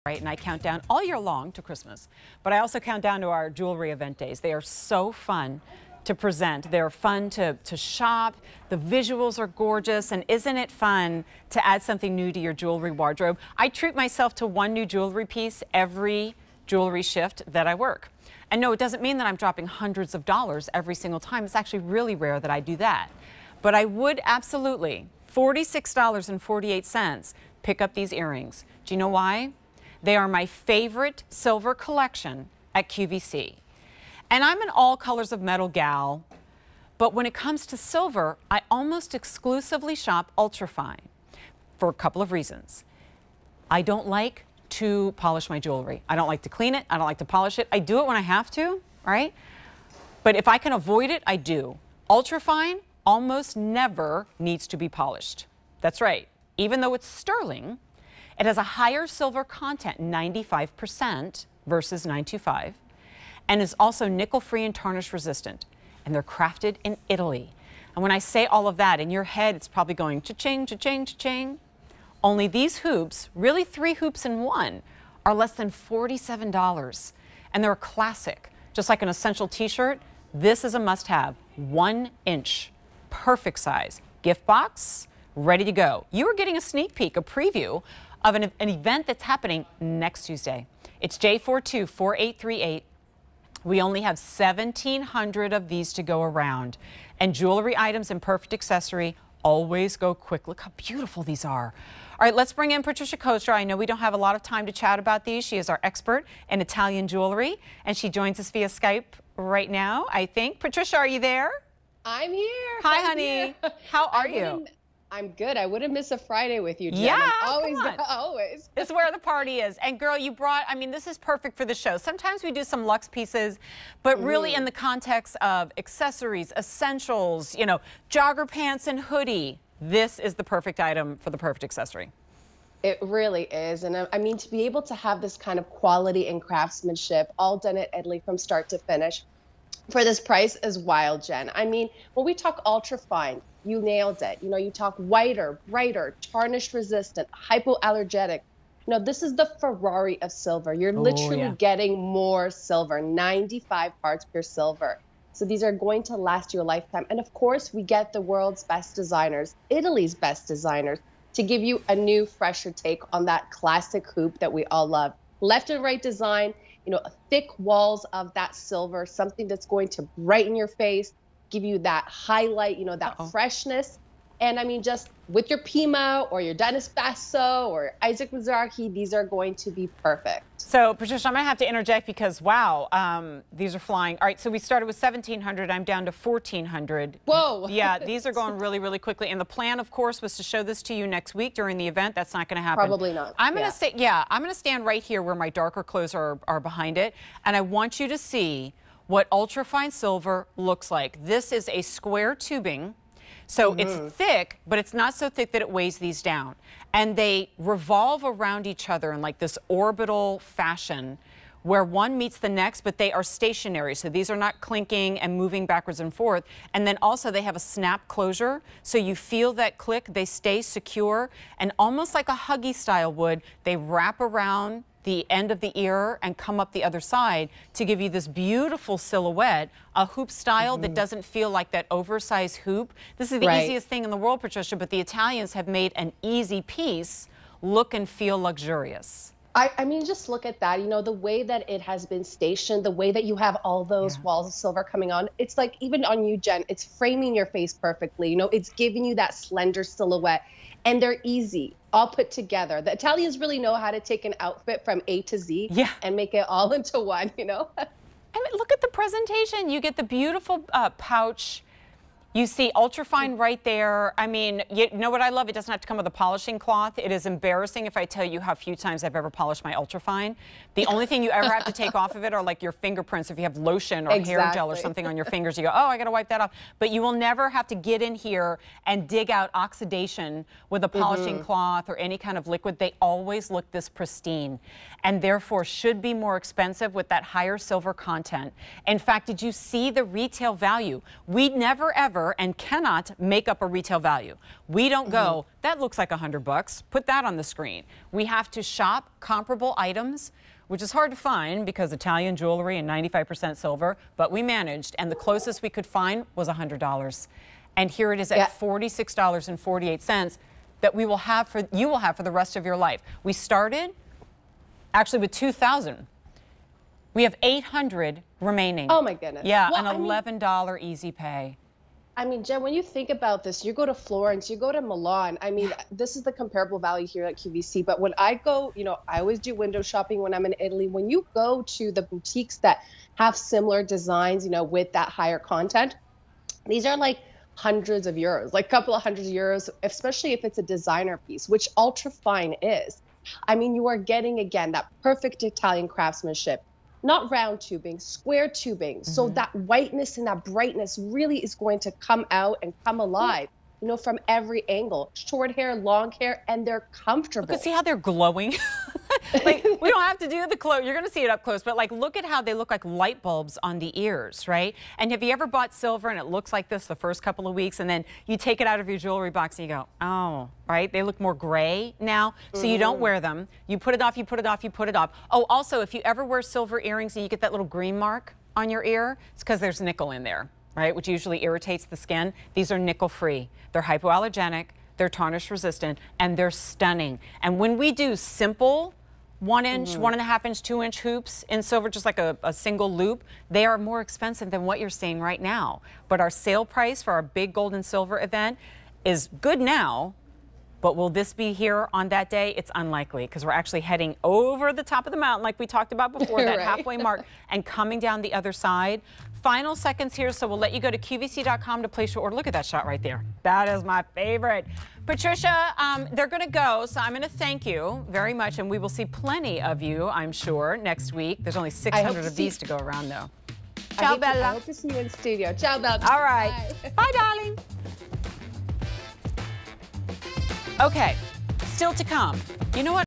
UltraFine Sterling Silver Triple Hoop 1" Earrings, 5.8g on QVC
UltraFine Sterling Silver Triple Hoop 1" Earrings, 5.8g

Treat yourself to elegance in triplicate with these sterling silver earrings that boast a polished and bolder presence than singular hoops. UltraFine&reg; 950 Silver Nickel-Free Jewelry.